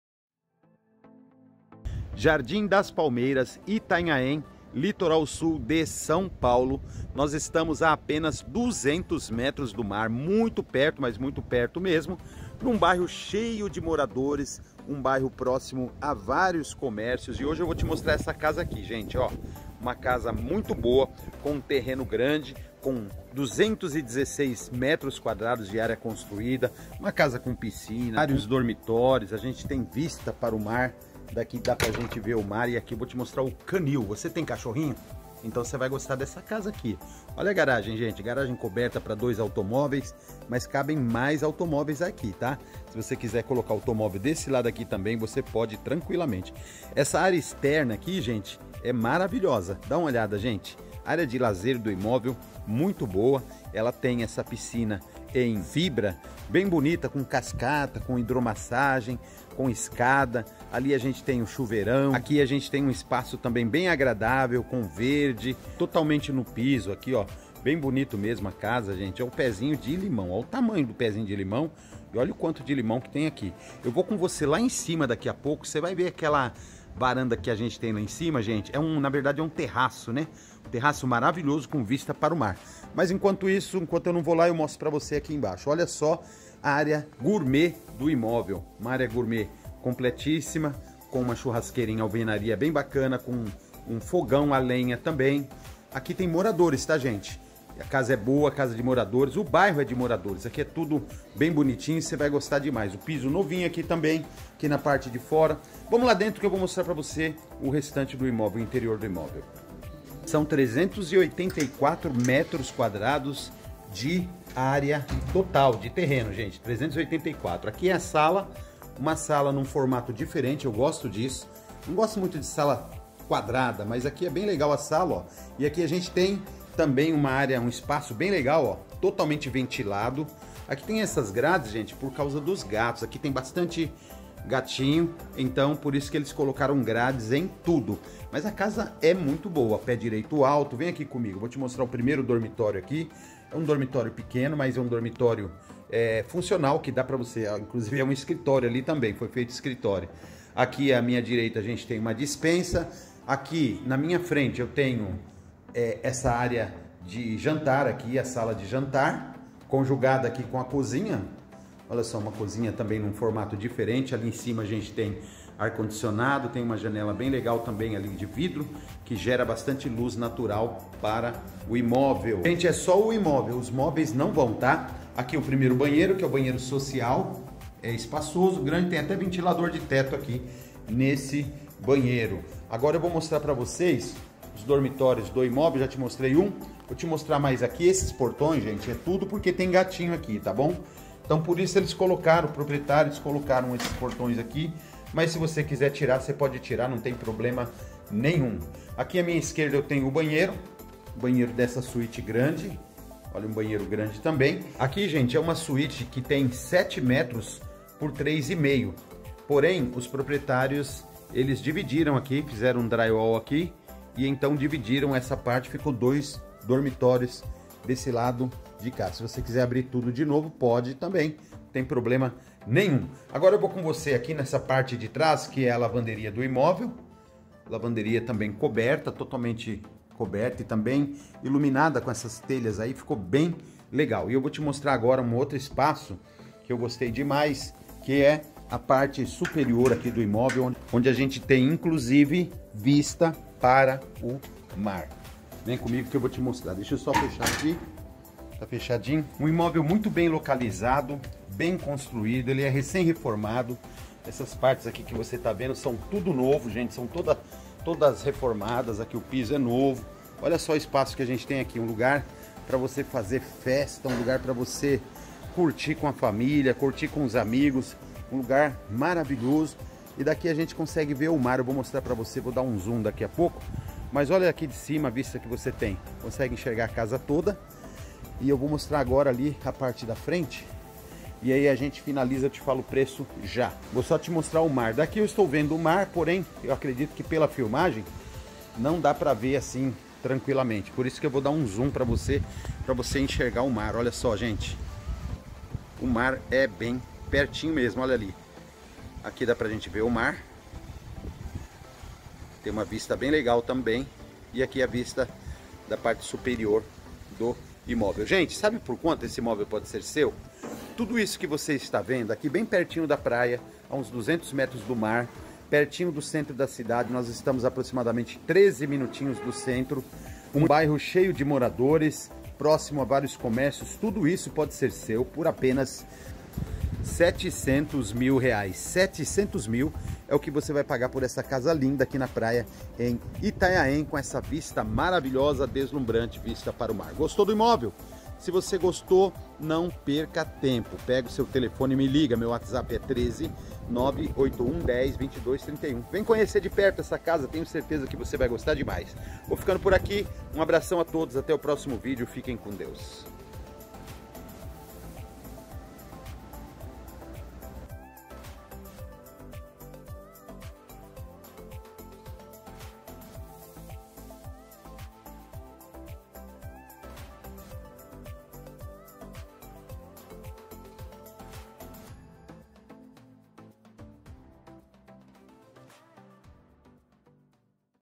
🏠 Casa na praia com piscina a 200 m do mar terreno grande confira!
👉 Muito espaço e muito conforto em um só lugar!
🚩Localização privilegiada, próximo a vários comércios, bairro residencial
REF:
⏰ Esse imóvel é ideal para você e sua família viverem com muito conforto e segurança, sabe porquê? O local é tranquilíssimo e tem acesso fácil à tudo o que você precisa! Fica á 12 min do centro de Itanhaém, perto de tudo: bancos, casas lotéricas, hospital, aeroporto, restaurantes, lojas cinema, supermercados como o Extra, Tenda, Atacadão e etc.

🚴‍♀️ As opções de lazer são inúmeras, fica próximo à orla, à pontos turísticos e ás praias mais lindas da cidade, bem como Gruta de Nossa Senhora de Lourdes, Cama de Anchieta, Morro do convento, Pocinho do Cibratel, Praia dos Sonhos, Morro do Paranambuco, praia do pescador onde foi filmado a novela Mulheres de Areia, cachoeiras, além de ser um local perfeito para passear, praticar esportes e admirar o pôr-do-sol.

👉 Você encontrará qualidade de vida, conforto, segurança e aconchego em um só lugar

❤️ 3 Dormitórios muito confortáveis
-  Salas de tv e estar grande muito arejada em conceito aberto
-  Cozinha maravilhosa com copa
- Vale a pena conhecer pessoalmente!
- 2 Banheiros grandes
- 🏊‍♀️ Piscina  com cascata, degraus e hidromassagem
🚕🚗🚙 Garagem para vários automóveis
-  Quintal  enorme ideal para quem possui pets
- Possui uma iluminação natural incrível!
- Iluminação em LED
-  Área de serviço coberta
- Portas em alumínio
- Portão automatizado
🌞 Deck e Solário maravilhosos
👨‍🍳 Espaço gourmet divino, completamente coberto com pia em granito,  e uma linda churrasqueira a carvão
🚿 Ducha fria

🧱📏 216,00 m² construídos de área total e 384,00 m² de área total

Uma ótima oportunidade para quem quer morar, investir, ou possuir um imóvel na Praia.
 REF:
✅ Tudo isso por apenas R$ 700.000,00 a vista, ou financiamento bancário em até 360 meses, podendo ser usado seu FGTS.

😄 Faça uma simulação gratuita sem compromisso,
ou Agende uma visita, você vai se surpreender!

😎  Itanhaém é uma cidade turística no litoral sul, de rara beleza, com vida própria e que vem crescendo dia após dia, com toda a infraestrutura de uma cidade litorânea de qualidade.
Agende sua visita e sinta na pele toda beleza e tranquilidade que esse lugar proporciona.